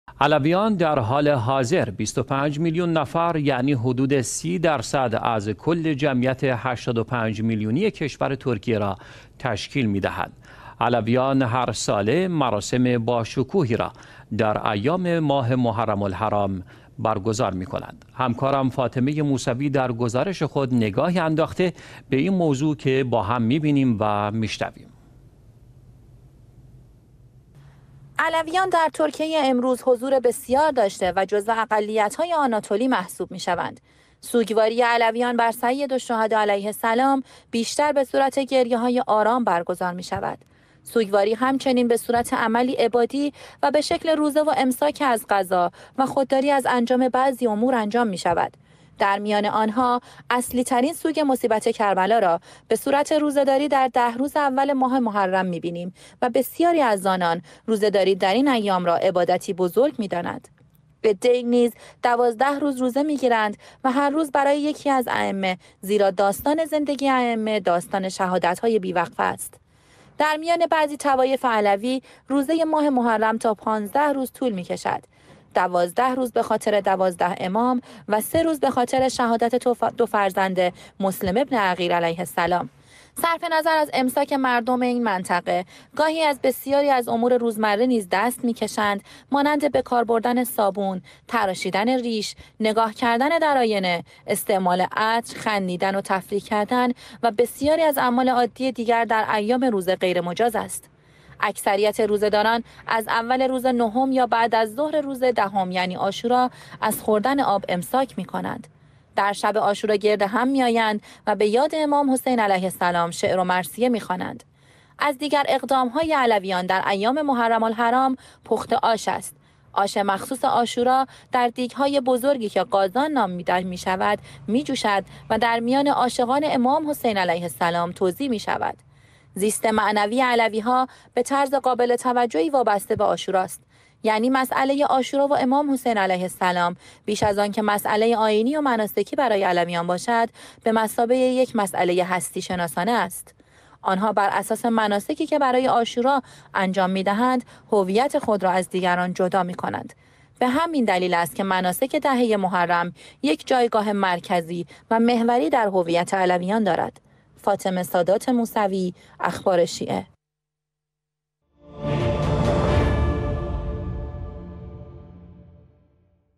نگاهی به مراسم ماه محرم در میان علویان ترکیه، فاطمه موسوی گزارش می دهد
علویان در حال حاضر ۲۵ ملیون نفر یعنی حدود ۳۰ % از کل جمعیت ۸۵ میلیونی کشور ترکیه را تشکیل می دهند.

علویان، هر ساله مراسم با شکوهی را در ایام محرم الحرام برگزار می کنند.

علویان، در ترکیه امروز، حضور بسیار داشته و جزو اقلیت‌های آناتولی محسوب می‌شوند.

سوگواری علویان بر سید الشهدا علیه السلام بیشتر به صورت گریه‌های آرام برگزار می‌شود.

سوگواری همچنین به صورت عملی عبادی و به شکل روزه و امساک از غذا و خودداری از انجام بعضی امور انجام می‌شود.

در میان آنها اصلی ترین سوگ مصیبت کربلا را به صورت روزه‌داری در ۱۰ روز اول ماه محرم می‌بینیم و بسیاری از آنان روزه‌داری در این ایام را عبادتی بزرگ می‌دانند.

عده‌ای نیز ۱۲ روز روزه می‌گیرند و هر روز برای یکی از ائمه؛ زیرا داستان زندگی ائمه داستان شهادت‌های بی‌وقفه‌ای است.

در میان بعضی طوایف علوی روزه ماه محرم تا ۱۵ روز طول می‌کشد؛ ۱۲ روز به خاطر ۱۲ امام و سه روز به خاطر شهادت دو فرزند مسلم بن عقیل.

صرف نظر از امساک مردم این منطقه گاهی از بسیاری از امور روزمره نیز دست می‌کشند؛ مانند به کار بردن صابون، تراشیدن ریش، نگاه کردن در آینه، استعمال عطر، خندیدن و تفریح کردن، و بسیاری از اعمال عادی دیگر در ایام روزه غیر مجاز است.اکثریت روزه‌داران از اول روز نهم یا بعد از ظهر روز دهم یعنی عاشورا از خوردن آب امساک می‌کنند.

در شب عاشورا، گرد هم می‌آیند و‌ به یاد امام حسین‌ علیه السلام شعر و مرثیه می‌خوانند.

از دیگر اقدام های علویان در ایام محرم الحرام، پخت آش است. آش مخصوص‌ عاشورا، در دیگ‌های بزرگی کـه «قـازان» می‌نامند، می‌جوشد و در میان عاشقان امام حسین علیه السلام توزیع می شود.

زیست معنوی علوی ها به طرز قابل توجهی وابسته به عاشورا است. یعنی مسأله عاشورا و امام حسین علیه السلام بیش از آنکه مسأله آیینی و مناسکی برای علویان باشد، به‌ مثابه یک مسأله هستی‌شناسانه است.

آنها بر اساس مناسکی که برای عاشورا انجام می‌دهند، هویت خود را از دیگران جدا می‌کنند. به همین دلیل است که مناسک دهه محرم، یک جایگاه مرکزی و محوری در هویت علویان دارد.